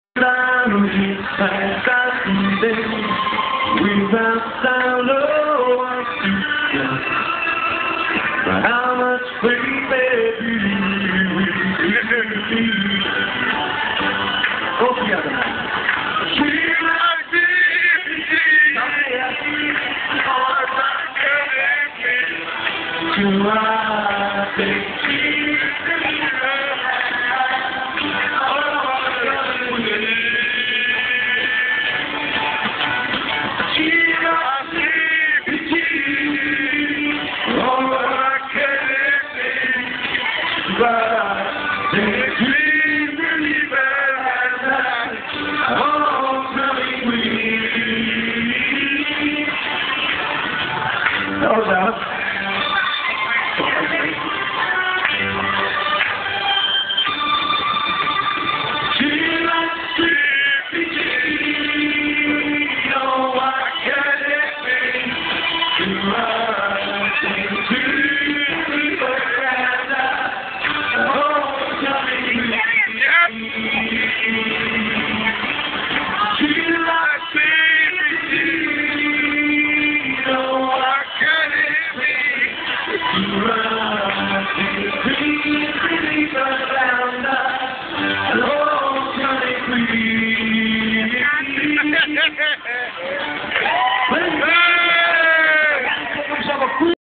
Damien shows you how to dance at a Nites2remember gig
damien gives it his all at a recent nites2remember gig at millbrook manor 2nd feb 2008